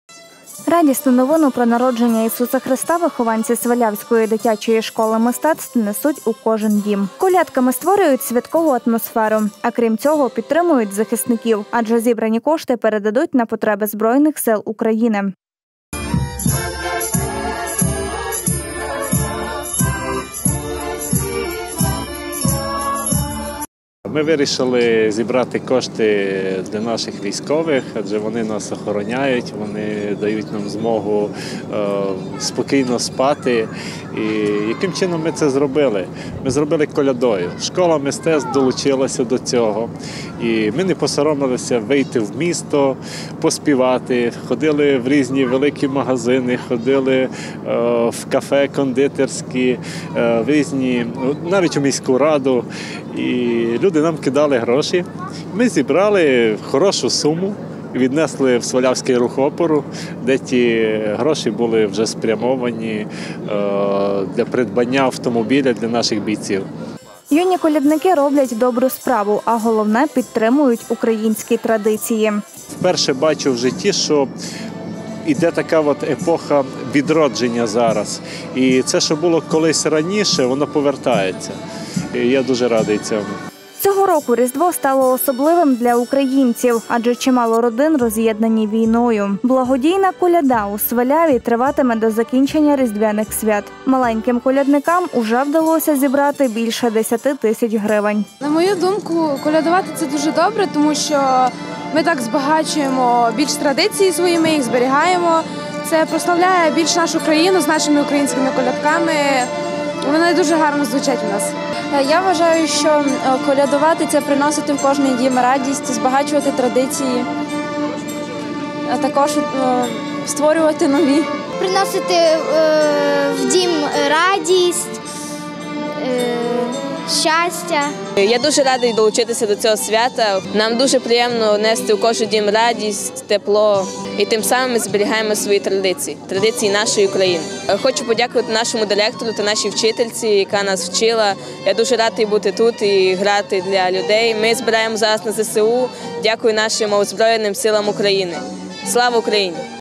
Благодійна коляда
Радісну новину про народження Ісуса Христа вихованці Свалявської дитячої школи мистецтв несуть у кожен дім. Колядками створюють святкову атмосферу, а крім цього підтримують захисників. Адже зібрані кошти передадуть на потреби Збройних сил України. Юні колядники роблять добру справу, а головне підтримують українські традиції.  Цього року Різдво стало особливим для українців, адже чимало родин роз’єднані війною. Благодійна коляда у Сваляві триватиме до закінчення різдвяних свят. Маленьким колядникам вже вдалося зібрати більше 10 000 гривень.